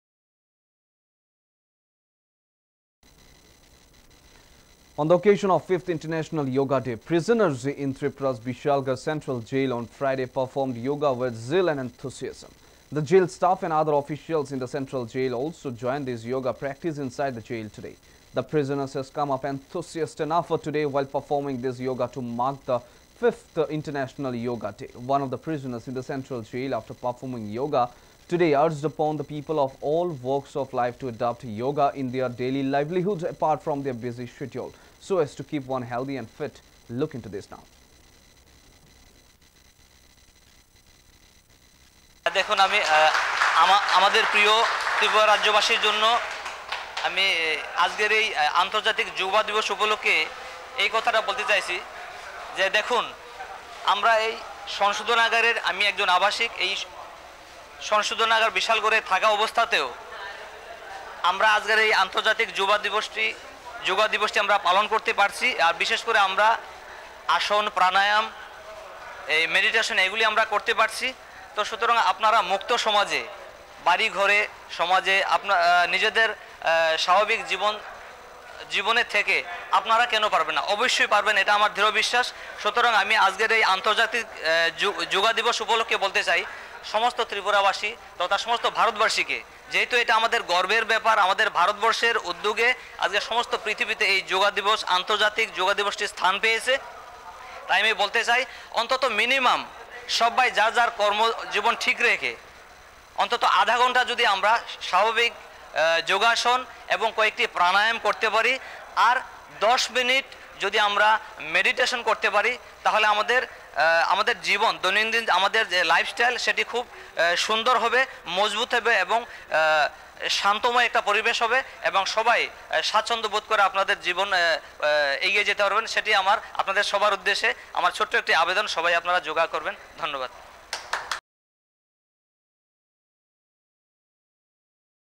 prisoners in Tripura's Bishalgarh central jail on Friday performed Yoga with zeal and enthusiasm.
On the occasion of 5th International Yoga Day, prisoners in Tripira's Bishalgarh central jail on Friday performed Yoga with zeal and enthusiasm.